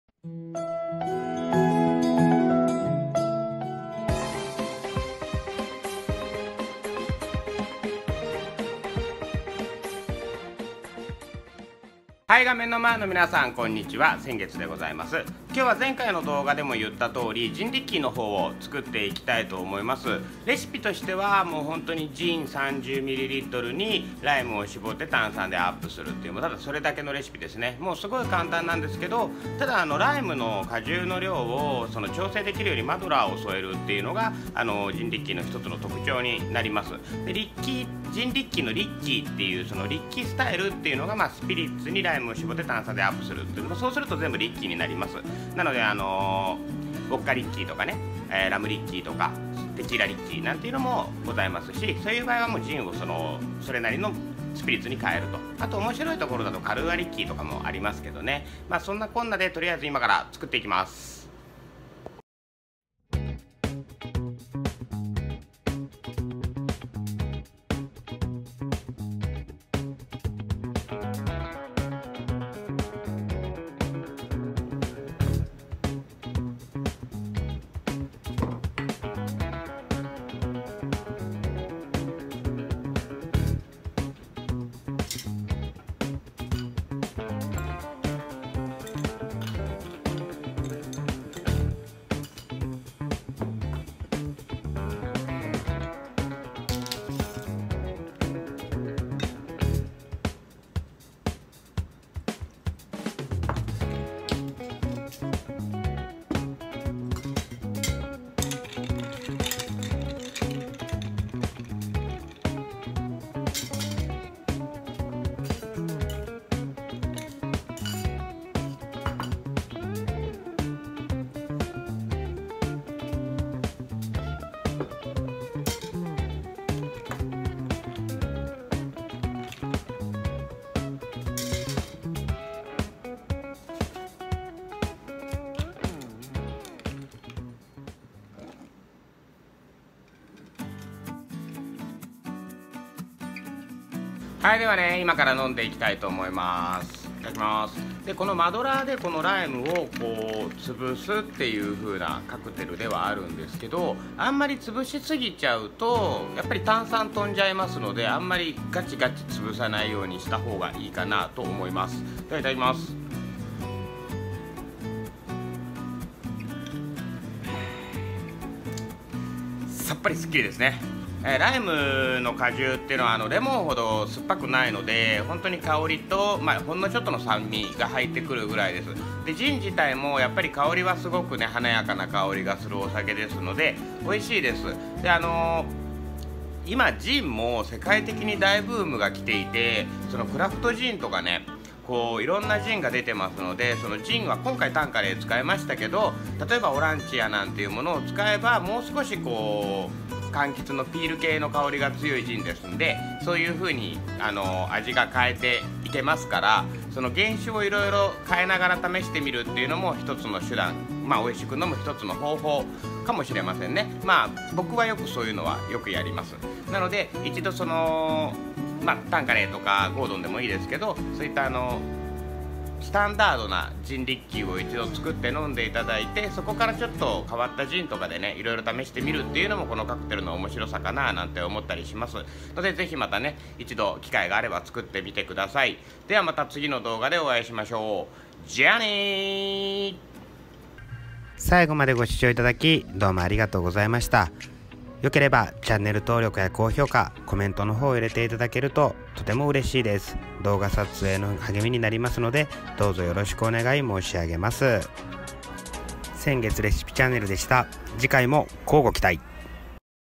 【おうちdeカクテル】簡単！！ジンリッキーの作りかた Easy! ! How to make Jin Ricky
愛知県豊橋市の和風BAR繊月です。今回は家でも作れる簡単、お手軽なカクテル。ジンリッキーのレシピ、そしてワンポイントの動画です。さっぱりしていて飲みやすい！！夏にゴクゴク飲めるカクテルですよ！！作りかたも物凄く簡単なので是非一度作ってみてください。
ジンリッキー( Jin Ricky)
recipe
ジン30ml(タンカレージン)
ライム1/6
炭酸でupする。
ライムはギューっと絞ると皮の苦味が出てしまうので注意！
炭酸は氷に当てないように入れてあげると良いですよ
ジンを変えると色々味が変化するので、自分の好きな1杯を探して見てください。